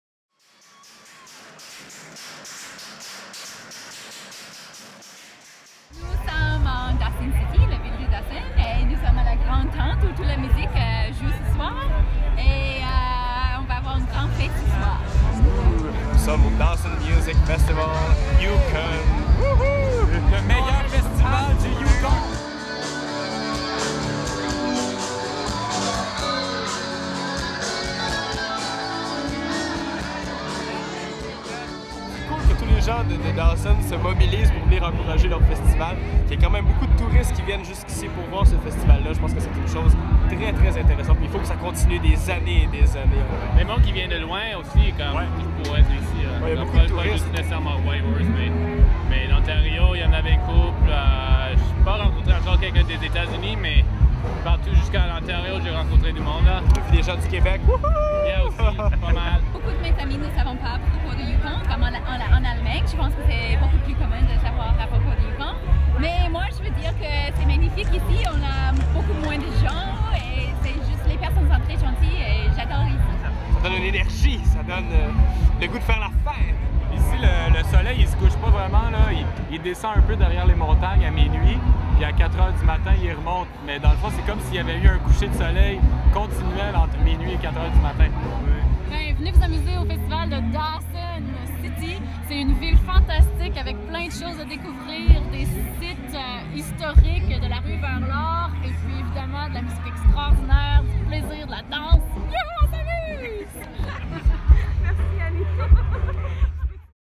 Festival de musique de Dawson - Yukon, Canada
Chaque été, les amateurs de musique au Canada se rassemblent pour un des plus important festival de musique se déroulant au nord du 60e parallèle, à savoir le Festival de musique de Dawson. Pendant trois jours, une féérie de sons fait vibrer la ville historique de Dawson. Quel que soit votre type de musique préféré vous y trouverez votre bonheur.

Pour planifier votre voyage au Canada visitez notre site !